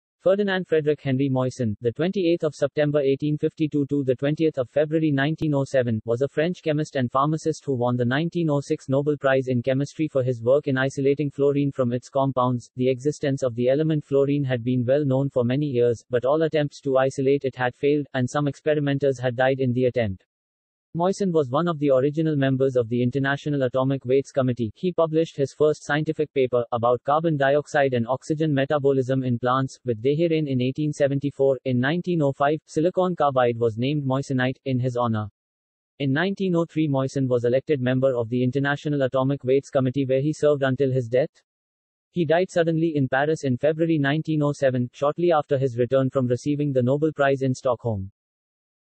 Tribute to Henri Moissan a French chemist and pharmacist who won the 1906 Nobel Prize in Chemistry
A Genuine Method to Generate Thousands of Dollars using FREE YouTube Videos Ferdinand Frédéric Henri Moissan (28 September 1852 – 20 February 1907) was a French chemist and pharmacist who won the 1906 Nobel Prize in Chemistry for his work in isolating fluorine from its compounds. The existence of the element fluorine had been well known for many years, but all attempts to isolate it had failed, and some experimenters had died in the attempt. Moissan was one of the original members of the International Atomic Weights Committee. He published his first scientific paper, about carbon dioxide and oxygen metabolism in plants, with Dehérain in 1874. In 1905, Silicon carbide was named moissanite, in his honor. In 1903 Moissan was elected member of the International Atomic Weights Committee where he served until his death. He died suddenly in Paris in February 1907, shortly after his return from receiving the Nobel Prize in Stockholm. His death was attributed to an acute case of appendicitis.